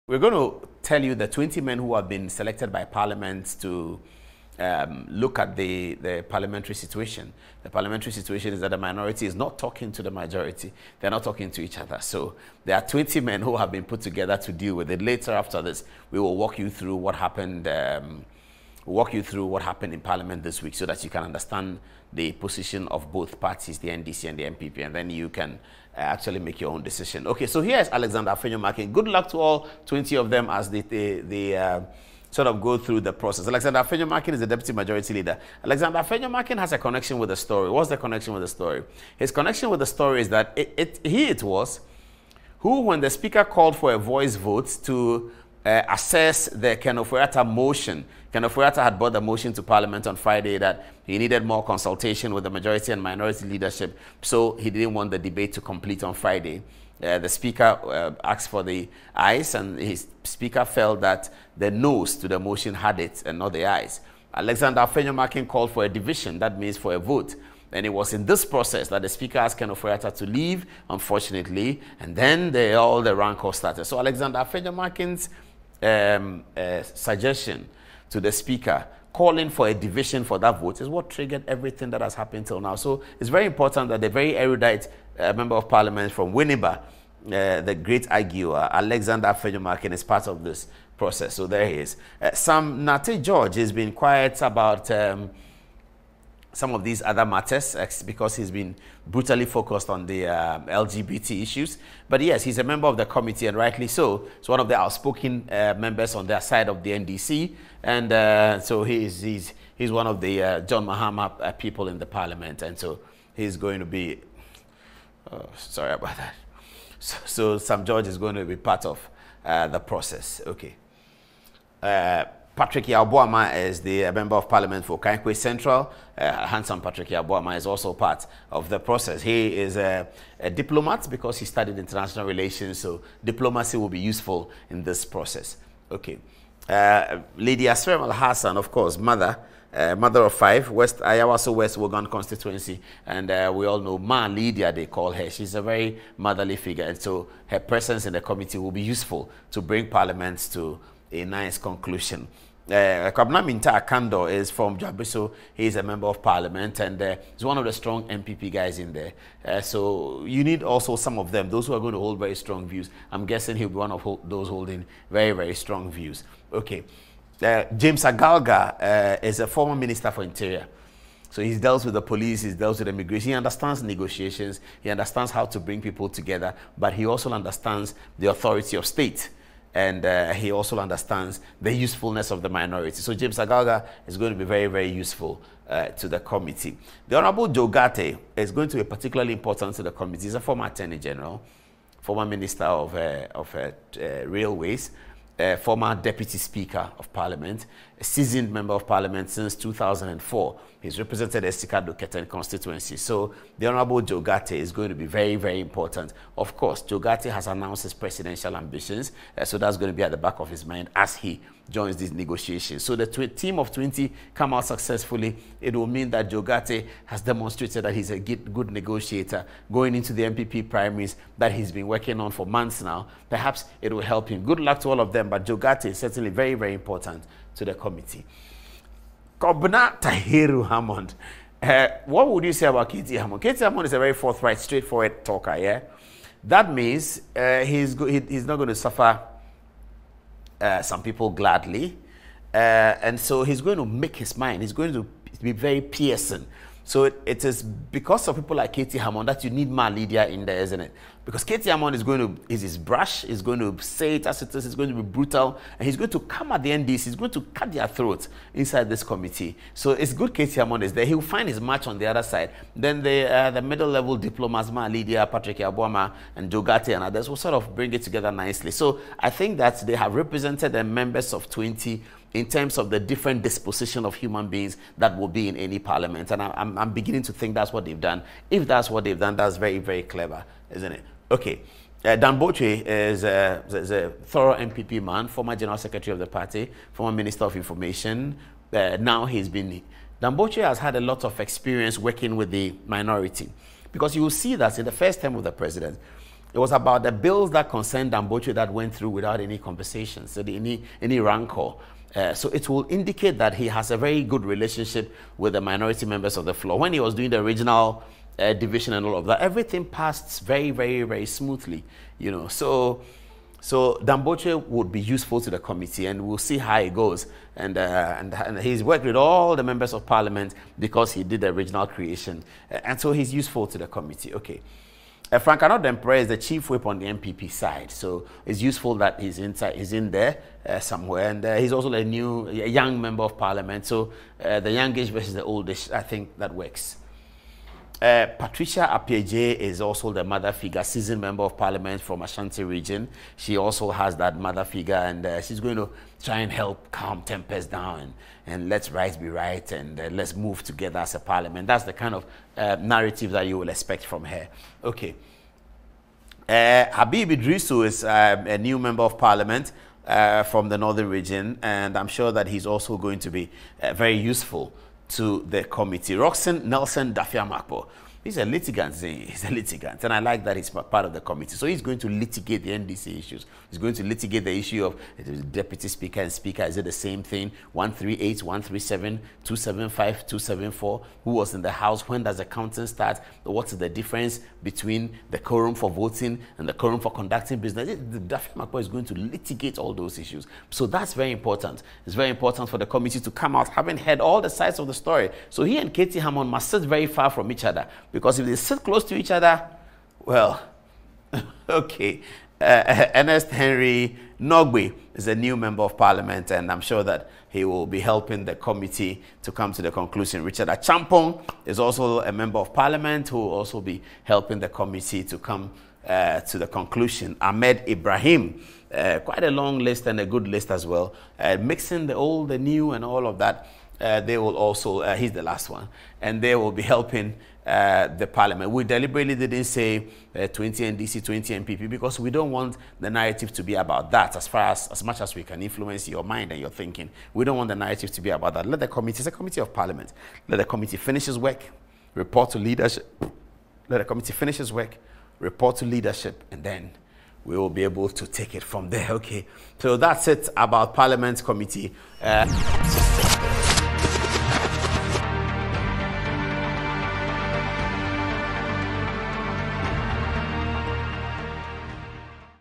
Parliament sets up a committee of 20 to look into issues after passing of the 2022 budget
This week, parliament set up a 20 member committee to look at the issues that have confronted the house following the passing of the budget... in this video we show you who the 20 members are and we assess their usefulness to the committee … Video... 7mins.

 @Paul Adom-Otchere